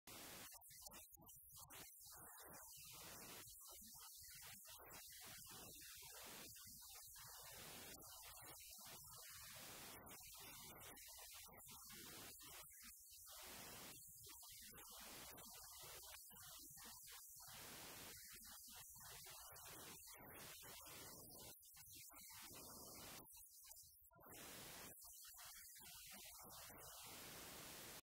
Spirit Fest Austin - Brandon Heath
Brandon Heath wants to see you at Dell Diamond on November 2nd for Spirit Fest 2013!